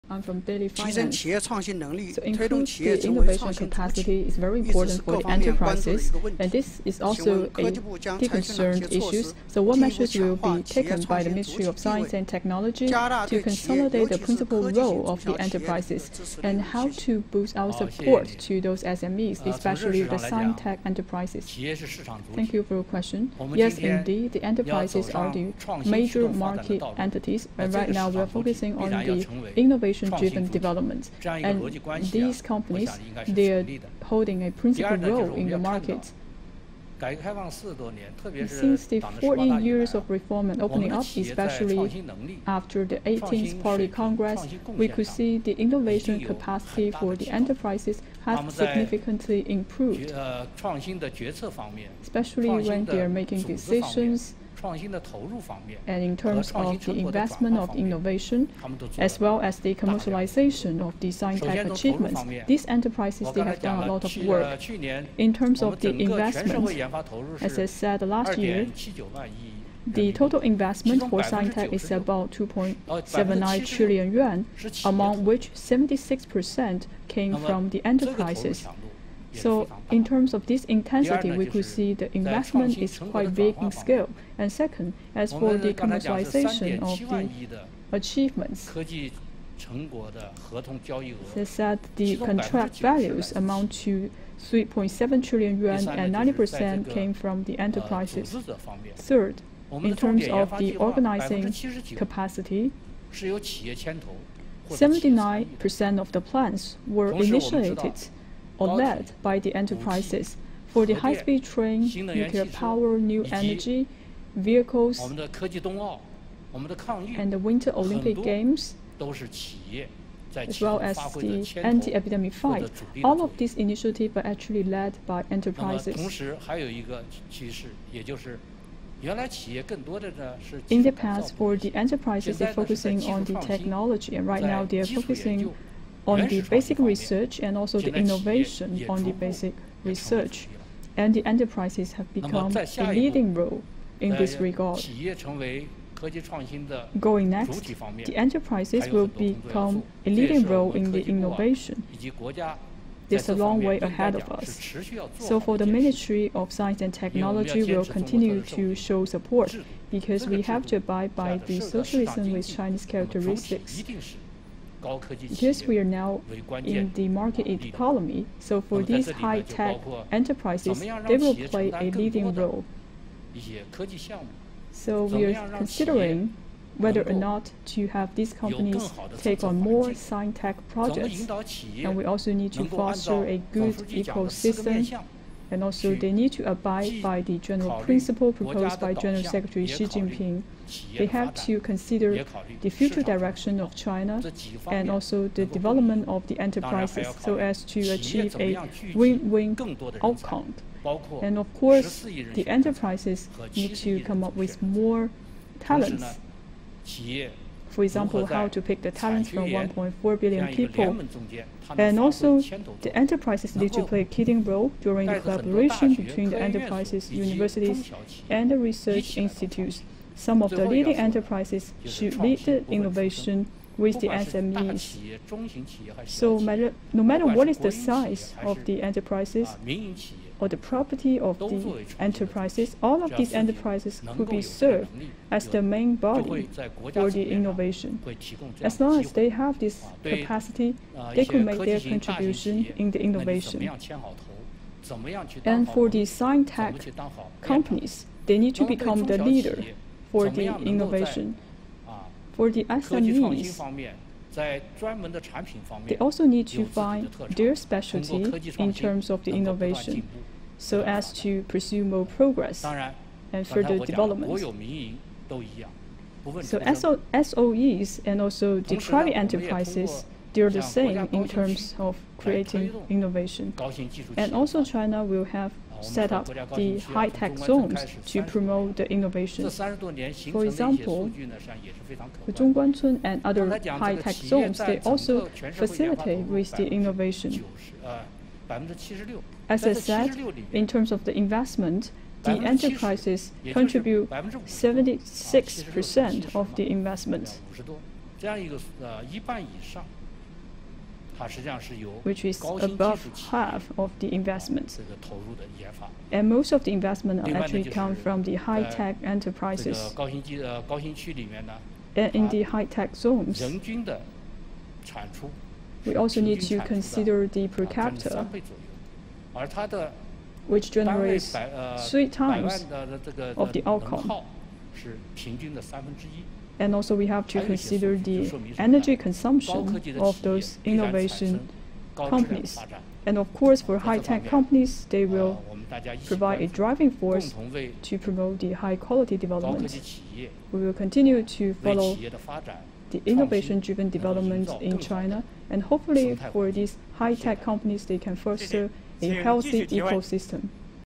China to focus on innovation-driven development for enterprises
China will focus on innovation-driven development for enterprises and create a better environment for them, said Wang Zhigang, Minister of Science and Technology, at a press conference after the second plenary meeting of the fifth session of the 13th National People's Congress (NPC) on Tuesday.